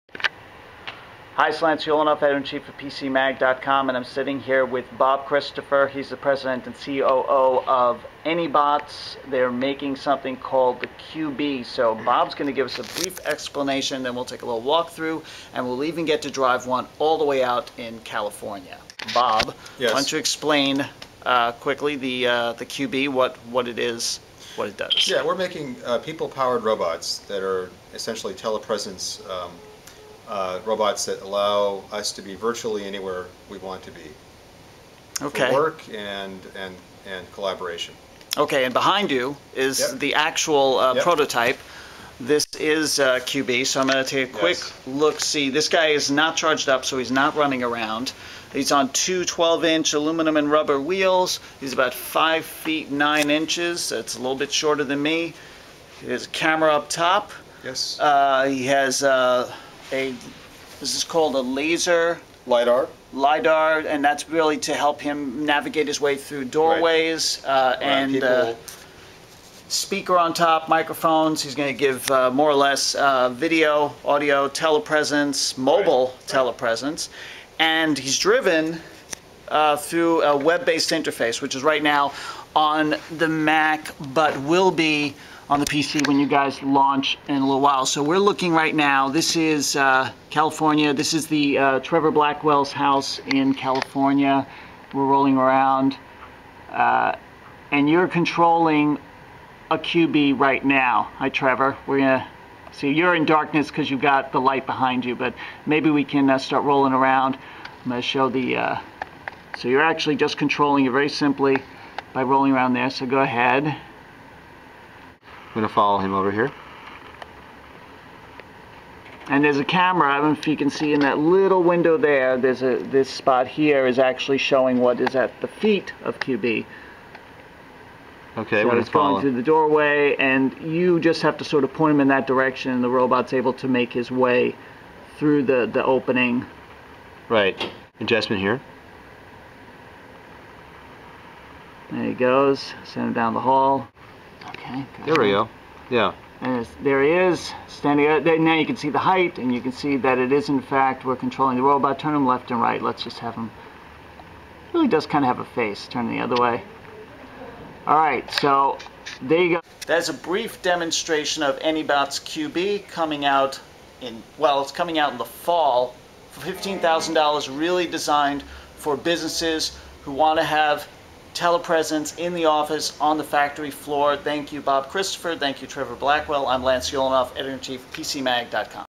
Up Close with Anybots QB Telepresence Robot
PCMag.com Editor-in-Chief gets up-close and personal with this new virtual person robot from Anybots. It's called QB and is remote-controlled via any PC.